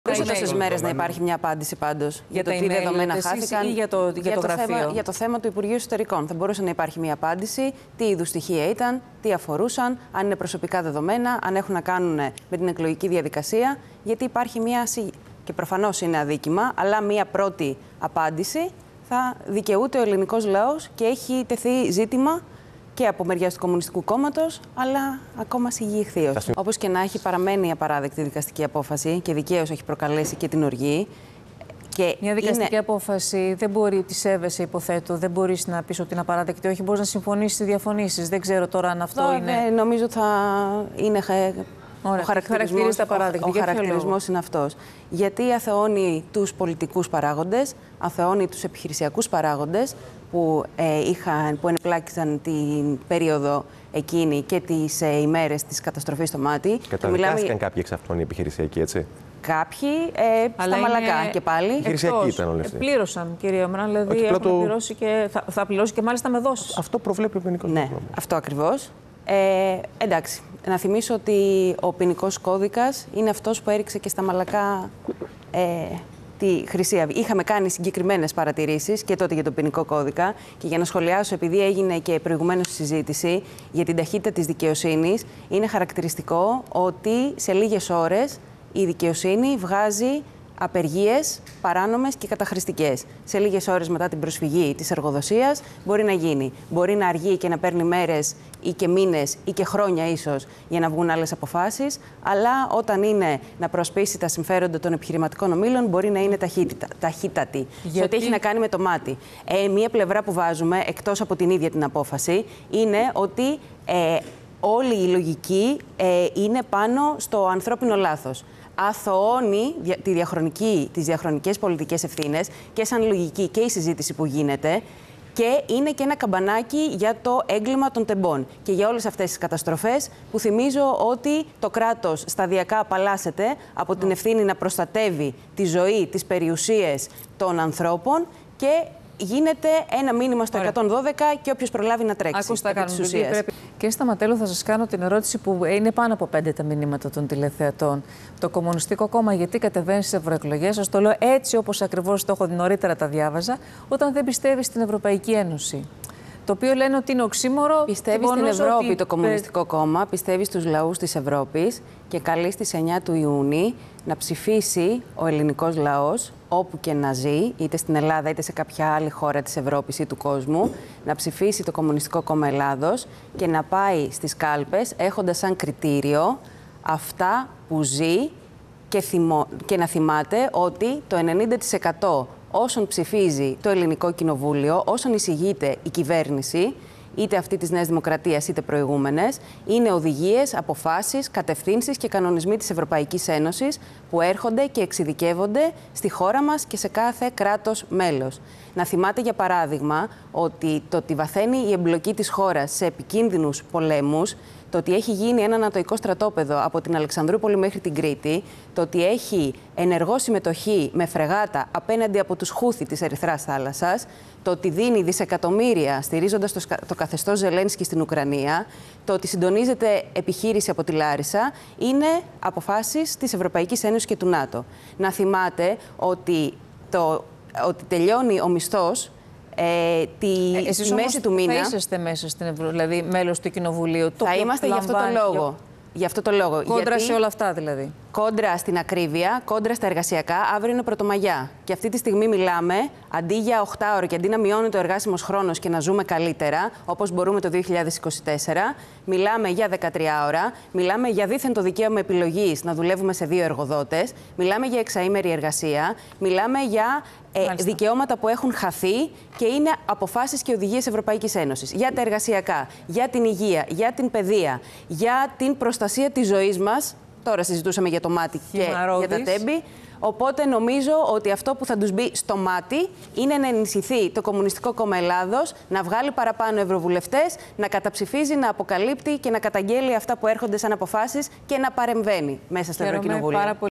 Δ. Σταματέλου: Το ΚΚΕ πιστεύει στη δύναμη των λαών της Ευρώπης
Η δικαστική απόφαση για τη φωτιά στο Μάτι είναι απαράδεκτη, γιατί αθωώνει τους πολιτικούς και επιχειρησιακούς παράγοντες που είχαν εμπλακεί εκείνη την περίοδο και συγκεκριμένα, στην καταστροφή που σημειώθηκε στην Ανατολική Αττική, τόνισε η υποψήφια ευρωβουλευτής του ΚΚΕ *Δήμητρα Σταματέλου,* μιλώντας στην ΕΡΤ.

Μάλιστα, η Δ. Σταματέλου στάθηκε και στο μεγάλο χρονικό διάστημα που χρειάστηκε για να ολοκληρωθεί η δίκη, επισημαίνοντας την επιλεκτική σβελτάδα που χαρακτηρίζει την αστική δικαιοσύνη, όταν, όπως είπε, προσφεύγει σε αυτήν η εργοδοσία, ζητώντας να κηρυχθούν παράνομες και καταχρηστικές απεργίες, οι σχετικές δικαστικές αποφάσεις εκδίδονται μέσα σε λίγες ώρες.

Ακόμα, πρόσθεσε ότι πρόκειται και για μια δικαστική απόφαση που βασίζεται στην άθλια λογική του «ανθρώπινου λάθους», την οποία ανέδειξε από την πρώτη στιγμή και ο ΣΥΡΙΖΑ και σιγόνταραν, από πλευρά τους, και η ΝΔ και το ΠΑΣΟΚ, αθωώνοντας, έτσι, διαχρονικές πολιτικές ευθύνες. Σε αυτό το σημείο, προειδοποίησε και για το τι μπορεί να σημαίνει η συγκεκριμένη δικαστική απόφαση και μελλοντικά, όπως στην περίπτωση του εγκλήματος των Τεμπών, μιας και εδώ η κυβέρνηση της ΝΔ μιλάει συνεχώς για «ανθρώπινο λάθος», ενώ ΣΥΡΙΖΑ και ΠΑΣΟΚ προσπαθούν να κρύψουν και τις δικές τους ευθύνες, επιρρίπτοντας ευθύνες για ζητήματα διαχείρισης και σε συγκεκριμένα πολιτικά και επιχειρησιακά πρόσωπα.

Επίσης, στην αρχή της συνέντευξής της, η Δ. Σταματέλου σχολίασε και το ζήτημα της διάρρηξης του υπουργείου Εσωτερικών, αναδεικνύοντας μια σειρά ερωτήματα που εγείρονται και εξακολουθούν να είναι αναπάντητα, όπως για το είδος των στοιχείων και τη συσχέτισή τους με την εκλογική διαδικασία. Υπογράμμισε ακόμα ότι ο ελληνικός λαός δικαιούται μια απάντηση για το συγκεκριμένο θέμα, τονίζοντας, ότι από κυβερνητικής πλευράς υπάρχει «σιγή ιχθύος»...

Τέλος, η υποψήφια ευρωβουλευτής του ΚΚΕ σημείωσε ότι το ΚΚΕ συμμετέχει στις Ευρωεκλογές γιατί πιστεύει στους λαούς της Ευρώπης και για αυτό καλεί τον ελληνικό λαό να πάει στην κάλπη με κριτήριο την ακρίβεια και την κατάσταση στα εργασιακά, την Παιδεία και την Υγεία, το γεγονός ότι η πλειονότητα όσων νομοθετούνται στο ελληνικό Κοινοβούλιο είναι ουσιαστικά αποφάσεις και οδηγίες της ΕΕ, ότι η πιο βαθιά εμπλοκή της χώρας σε επικίνδυνους πολέμους είναι, επίσης, αποφάσεις του ΝΑΤΟ και της ΕΕ και να στηρίξει, τελικά, το ΚΚΕ στις 9 Ιούνη: «Αυτό που θα τους μπει στο μάτι είναι να ενισχυθεί το ΚΚΕ, να βγάλει παραπάνω ευρωβουλευτές, να καταψηφίζει, να αποκαλύπτει, να καταγγέλλει και να παρεμβαίνει».